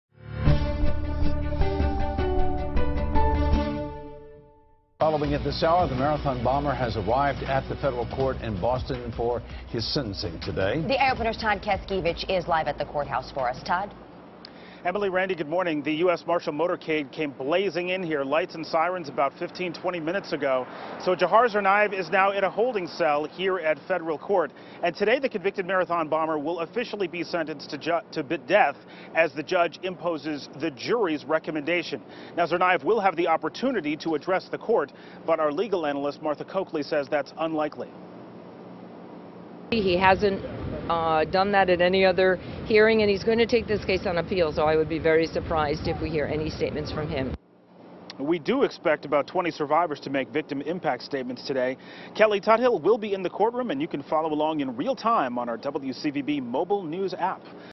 Will bomber finally speak as death sentence is imposed?
Lawyers for Boston Marathon bomber Dzhokhar Tsarnaev have said he feels remorse for the deadly attack, but the public has never heard directly from him.